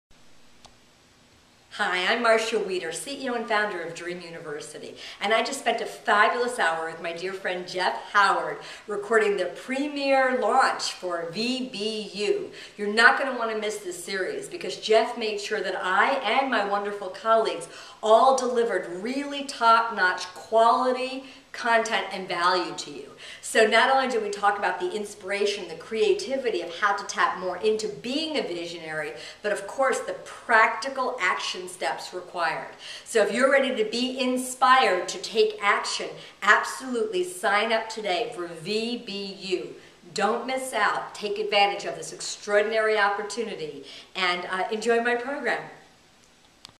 Marcia Wieder on Visionary Business University
-- Marcia Wieder is one of the expert faculty at Visionary Business University, speaking on the topic of "12 Steps to Being a 21st Century Visionary".  Marcia and 23 other visionaries will be sharing their knowledge, strategy and wisdom with you, at no charge starting September 13.  Tons of great preparation content available before the event.